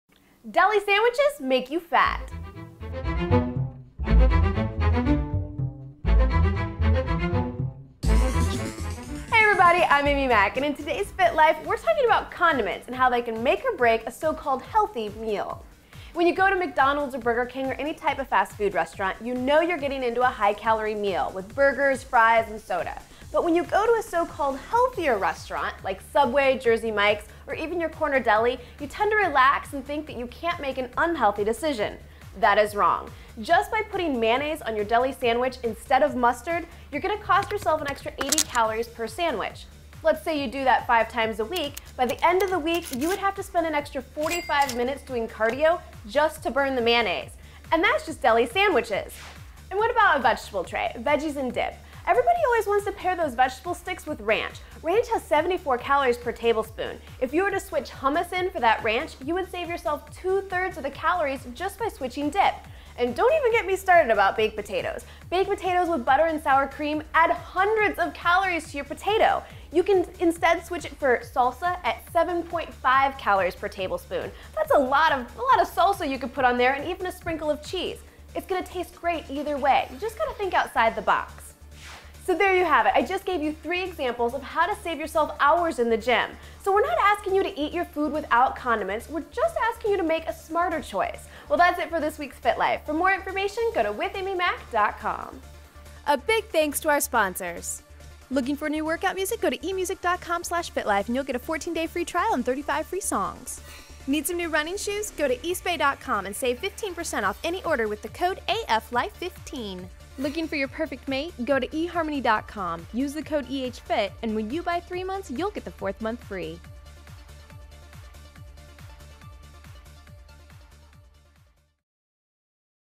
Deli Sandwiches Make You Fat?!
Everyday you have to face lunchtime. What are you going to eat? How healthy is it? How over-sized will the portion be? Sometimes, you make a healthier choice but you are still sabotaging yourself because you aren't practicing condiment caution. In this Fit Life, we take a look at some condiment alternatives to help you keep your diet in check when eating deli sandwiches, baked potatoes and veggie trays. Find other fitness tips, exercise videos and easy recipes